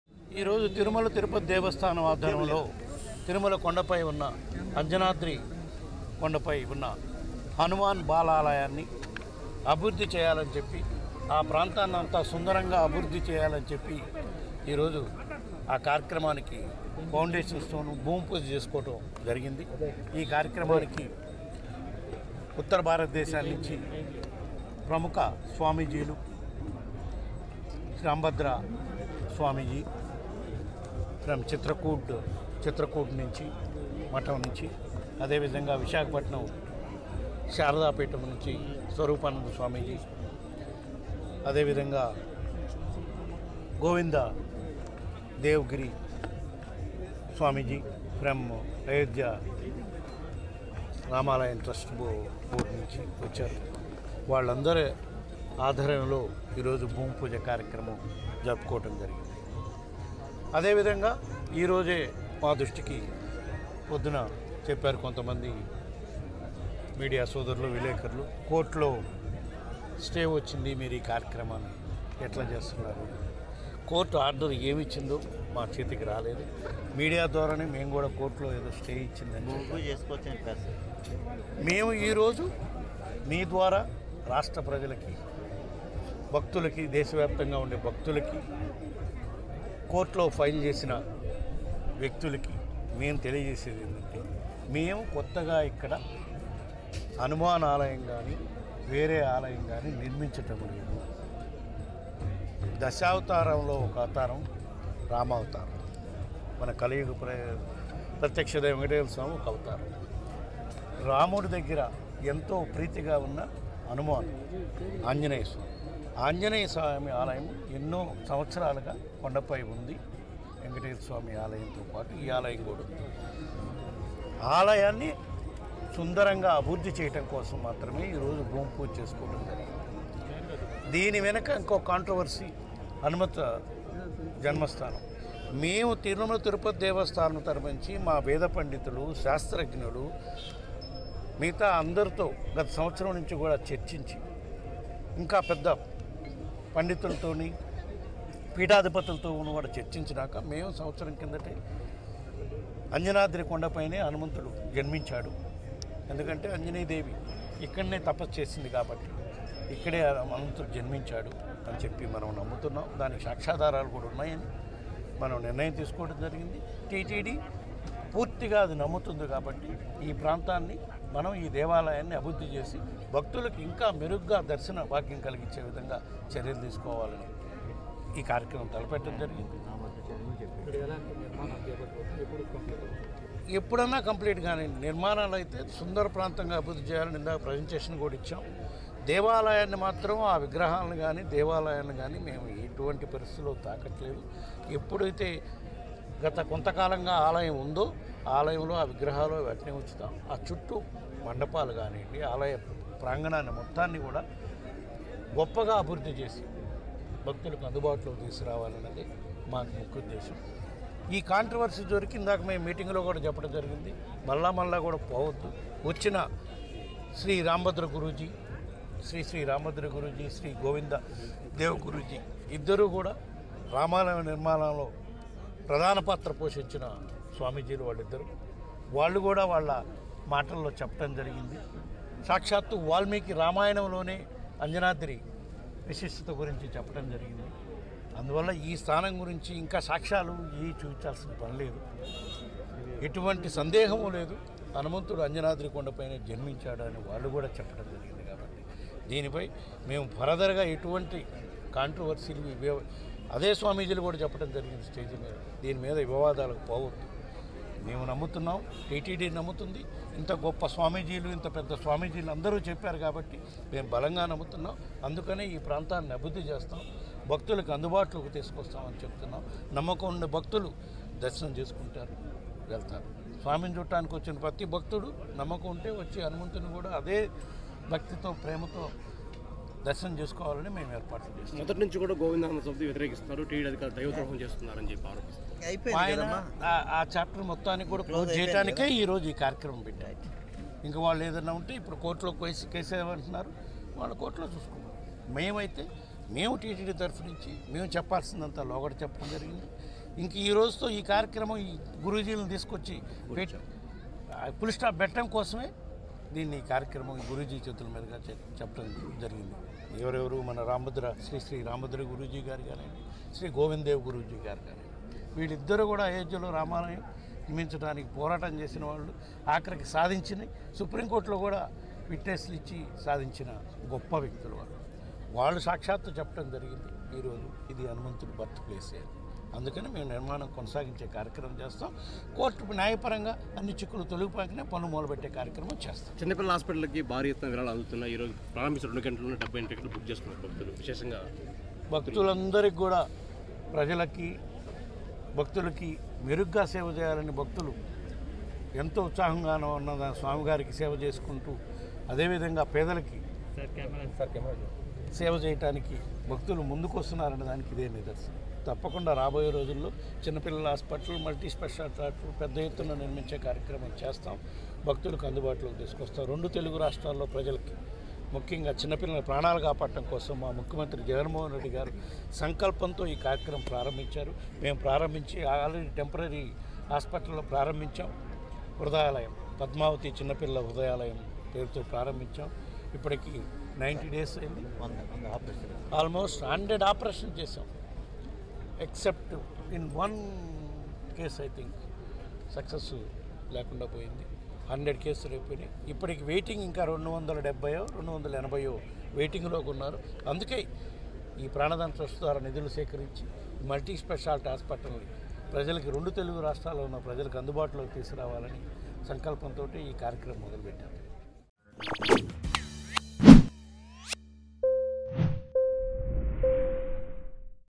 Hanuman birthplace: హనుమంతుడు తిరుమలలోనే జన్మించాడని టీటీడీ నమ్ముతోంది: వై.వి.సుబ్బారెడ్డి I TTD
Hanuman birthplace : హనుమంతుడు తిరుమలలోనే జన్మించాడని టీటీడీ నమ్ముతోంది: వై.వి.సుబ్బారెడ్డి

తిరుమల: అంజనాద్రిలో ఆలయ అభివృద్ధి కార్యక్రమం చేసుకోవడం అభినందనీయమని టీటీడీ చైర్మన్ వై.వి.సుబ్బారెడ్డి అన్నారు. బుధవారం మీడియాతో మాట్లాడుతూ హనుమ జన్మస్థలంపై కొందరు వివాదాస్పదం చేస్తున్నారన్నారు.